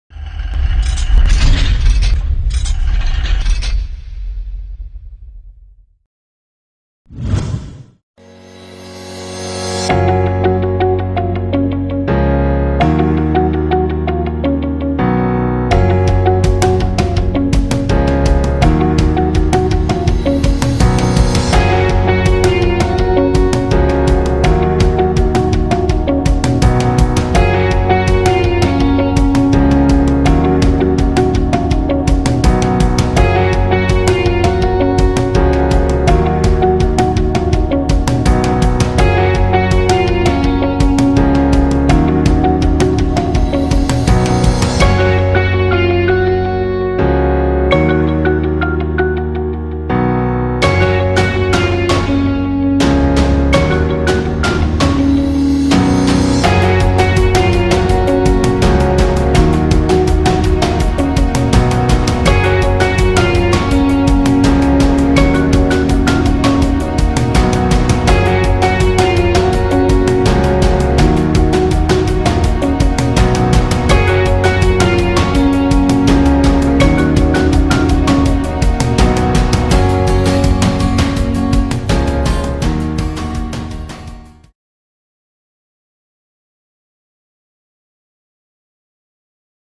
rundutec® | PKW Backup Stützbatterie by GÖHRUM Fahrzeugteile GmbH
rundutec® | PKW Backup Stützbatterie

rundutec BUP9 und BUP14 sind Versorgungs-/Stützbatterien für PKW in modernster AGM-Technologie. Völlig Wartungsfrei zur Unterstützung der Bordelektronik.

▬ GÖHRUM ▬▬▬▬▬▬▬▬▬▬▬▬▬▬▬

▬ Die Fakten ▬▬▬▬▬▬▬▬▬▬▬▬▬▬

EIN TEIL DES ERFOLGS:
- Handelstreue
- klare Sortimentspolitik

GÖHRUM Fahrzeugteile GmbH
Ulmenstr. 19
71069 Sindelfingen

▬ Ende ;-) ▬▬▬▬▬▬▬▬▬▬▬▬▬▬▬